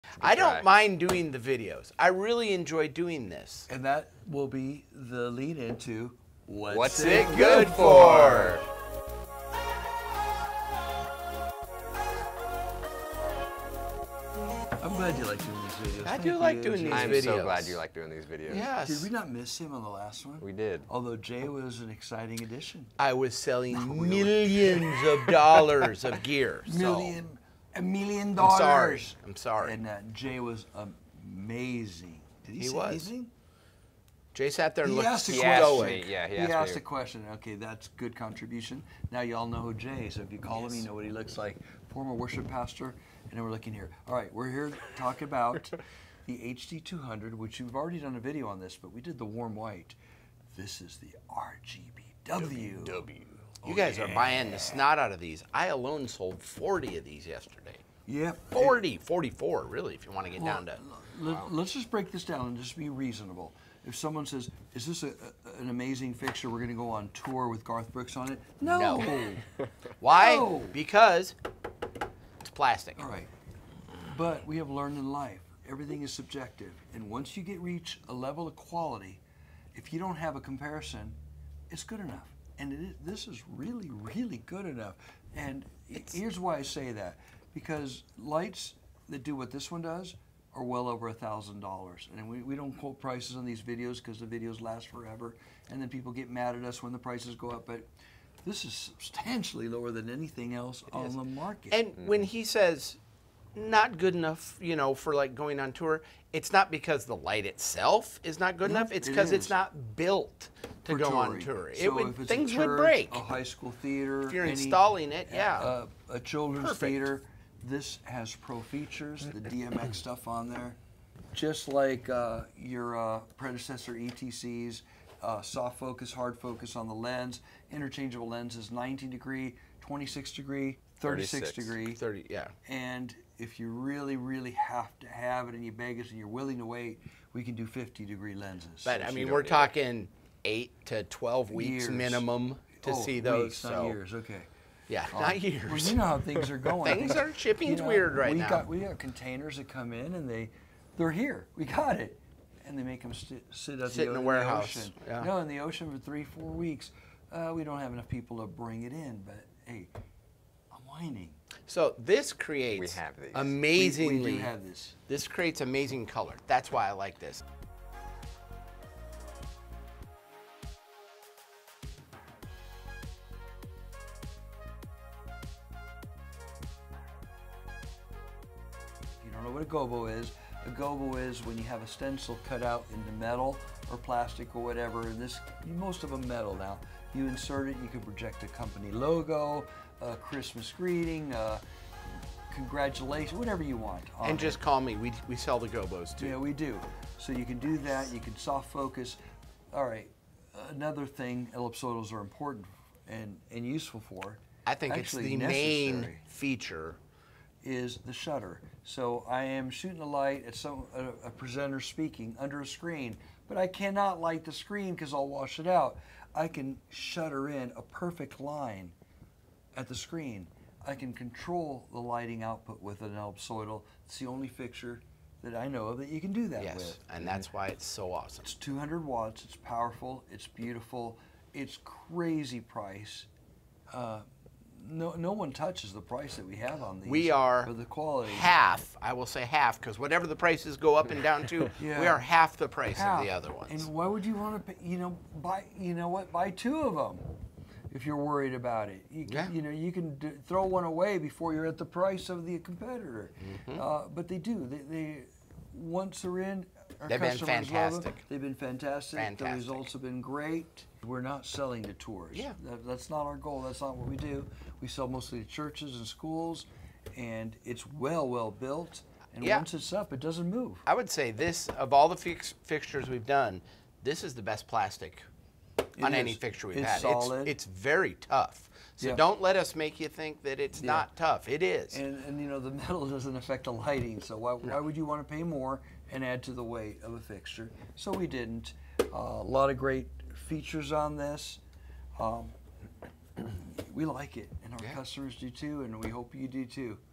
RGBW LED Ellipsoidal -200Watts | Economy Series
A 200Watt LED Ellipsoidal that produces thousands of vibrant colors at an output comparable to a traditional 750Watt ellipsoidal fixture.